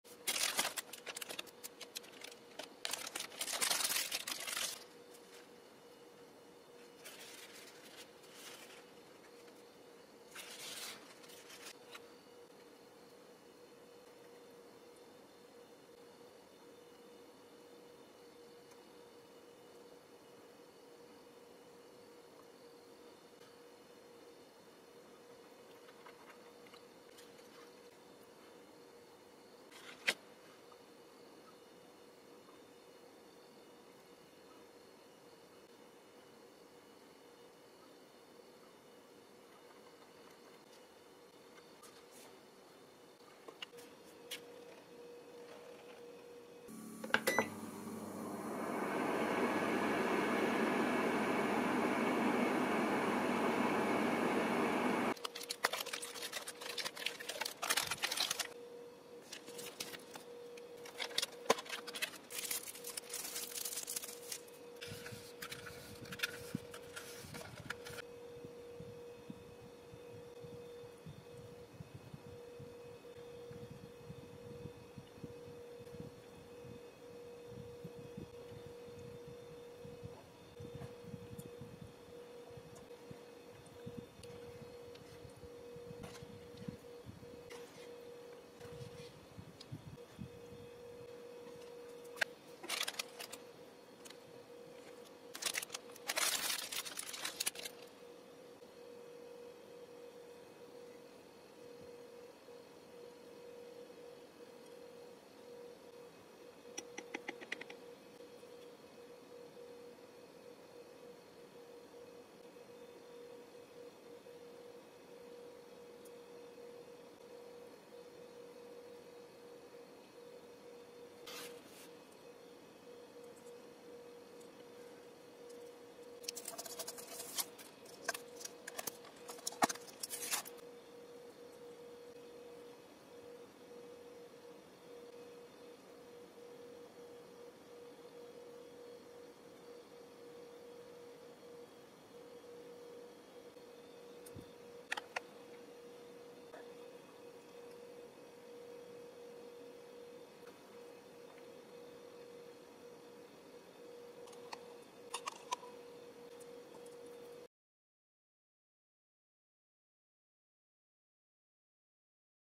【Japanese Convenience Store】【LAWSON】Onigiri Set, Akihabara Ramen and New Japanese Sweets
This channel introduces Japanese food.